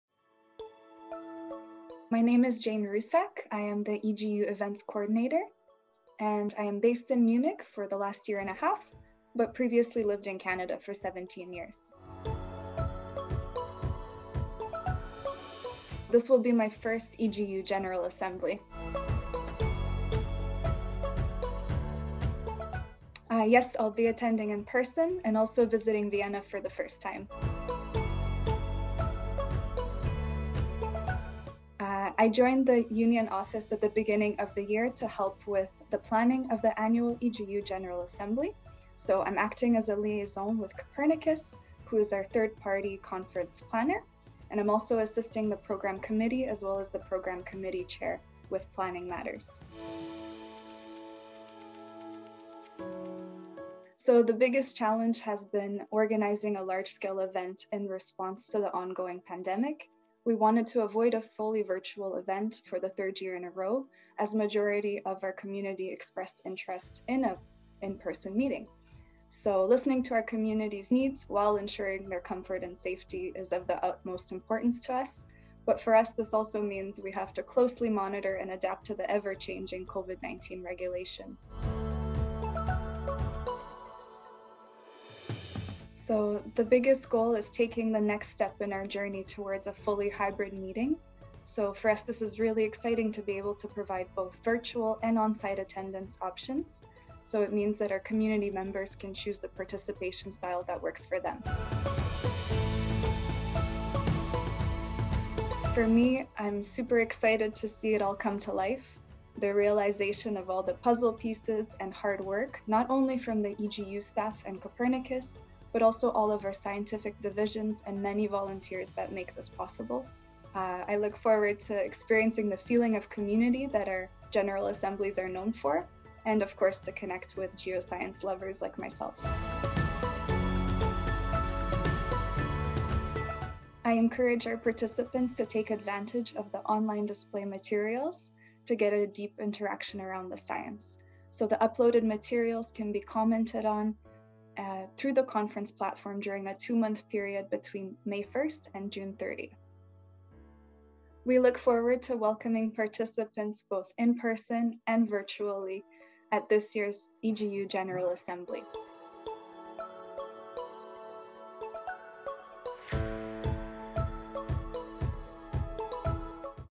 EGU22 - WeAreEGU Jane Roussak
Meet Jane Roussak, EGU Events Co-ordinator and first-time General Assembly attendee. In this short interview, she gives us an insight into the challenges of organising a hybrid EGU General Assembly for the first time in its history!

If you come across Jane during EGU22, why not say hello!

WeAreEGU is a new video series created by our two EGU22 Communications Interns; Fabio Crameri and Ubwiza Chiyungi.